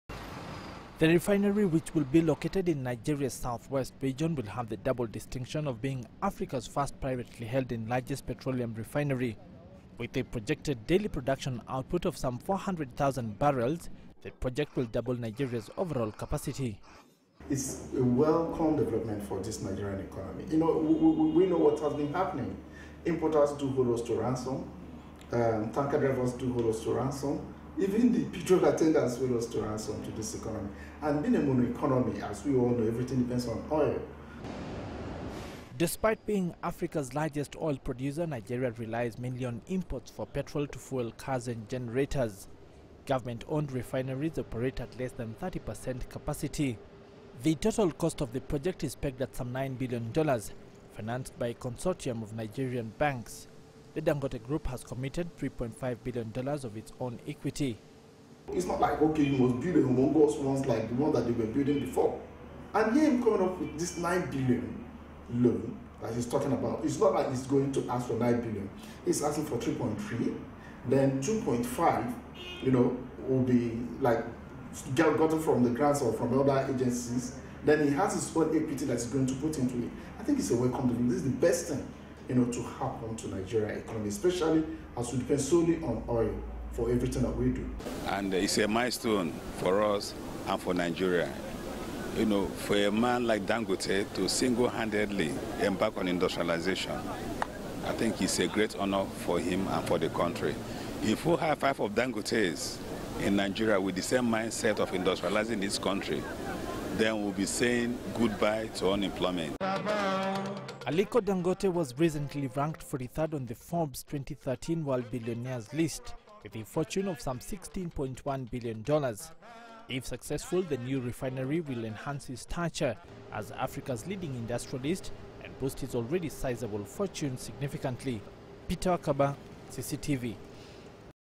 Africa's richest man, Dangote, to build oil refinery
Africa's richest man and Chairman of the Dangote Group of companies, Aliko Dangote, recently outlined plans to borrow $ 3.3 B to partly fund the construction of a 9-billion dollar oil refinery and petrochemical complex in Nigeria. Currently, the group has interests ranging from cement and basic food processing to oil and gas. If Dangote's multi-billion dollar bet goes according to plan, Nigeria could be less dependent on fuel imports than it is right now, as Peter Wakaba reports.